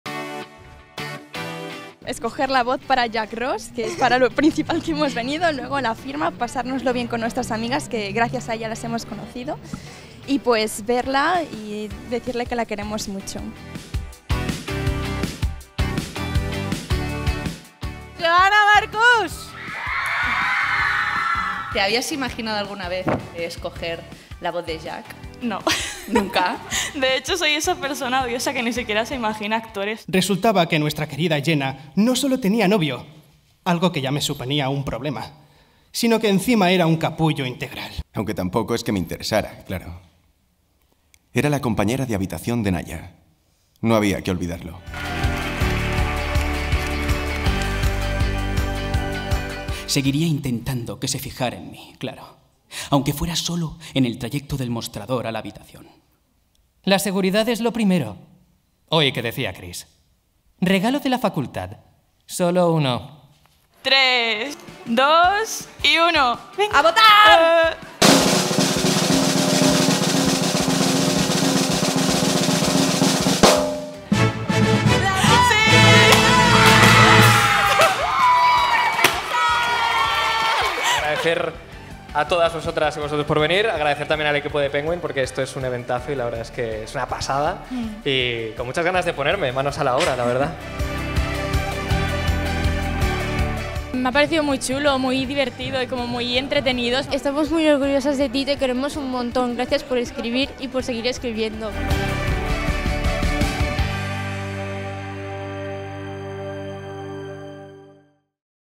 Joana Marcús y sus lectoras eligen la voz de Jack Ross en directo | Audiolibro «Tres meses»
¿No pudiste asistir al evento de «La voz de Jack Ross»? Recuerda con nosotros cómo Joana Marcús y sus lectoras eligieron, en el primer casting de voz en directo para un audiolibro en español, la voz de Jack Ross para el audiolibro de su nueva novela: «Tres Meses».

La esperada tercera parte de la saga Meses a tu lado de Joana Marcús. Jack Ross no creía en el compromiso, y hasta ahora no le había ido mal. La fantasía del amor verdadero, de ese cruce de miradas que te cambia la vida por completo, no era más que un argumento sobreexplotado de las películas que más solía criticar.
Por eso, ¿qué importaba que hubiera conocido a Jenna? ¿Qué importaba si pasaban tanto tiempo juntos? ¿Qué importaba si, poco a poco, iba abriéndole su corazón? Y, sobre todo, ¿qué importaba el riesgo de que se rompiera?
En tres meses pueden ocurrir muchas cosas, y enamorarse es una de ellas.
Jack Ross no iba a ser menos.